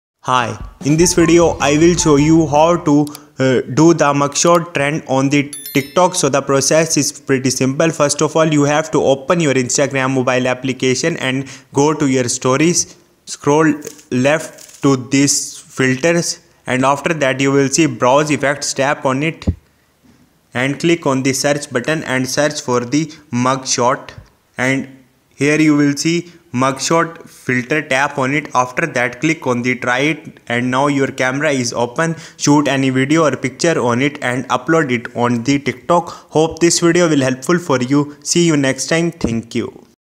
How To Do The Mugshot Trend On TikTok
Learn How To Do The Mugshot Trend On TikTok

In this video I will show you how to do mugshot trend on TikTok. This will be a short guide that shows you exactly how to do the mugshot trend.

Here's how to do mugshot TikTok trend.